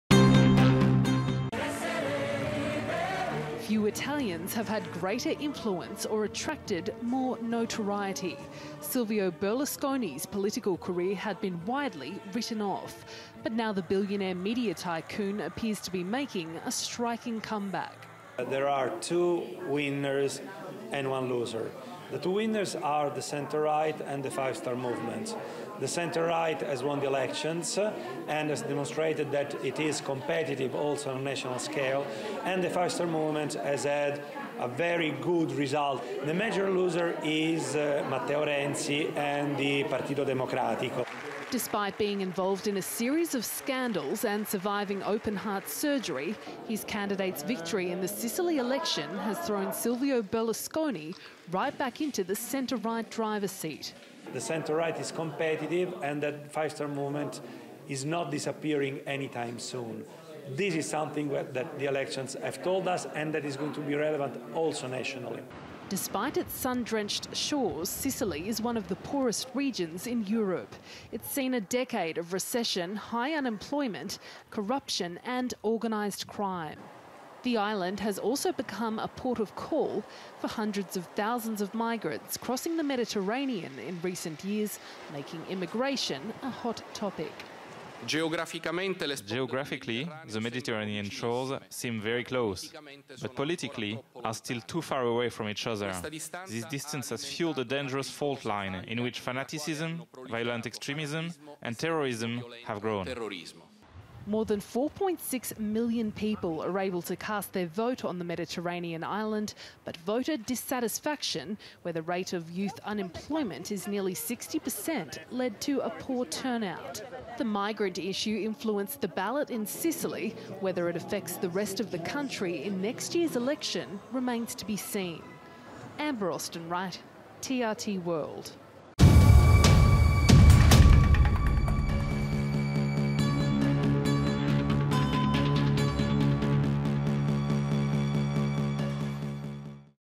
Italian Politics: Berlusconi likely to turn back to centre-right
Italy's former Prime Minister, Silvio Berlusconi, is poised for a political comeback. His coalition has claimed victory in a regional election in Sicily, and while a conviction for tax fraud means he's barred from running in upcoming national elections, the 81-year-old is going to the European Court of Human Rights to try to have the ban overturned. Amber Austin-Wright reports.